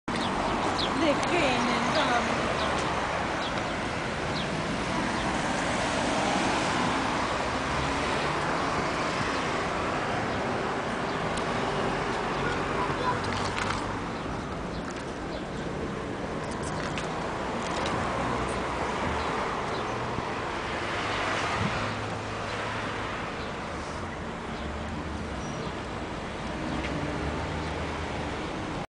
2011.03.16 Catel la Piata Sudului
Un cățeluș în zona Big Berceni, Piața Sudului, București, 16 martie 2011. Detalii